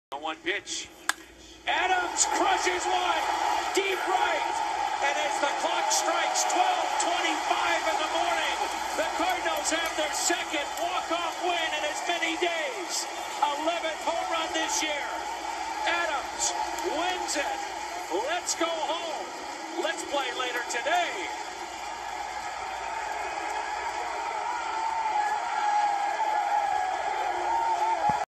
Matt Adams walk-off HR in 16th: 7/22/16
St. Louis Cardinals Matt Adams crushes a walk-off solo home run in the bottom of the 16th inning to give the Cardinals a 4-3 victory of the Dodgers.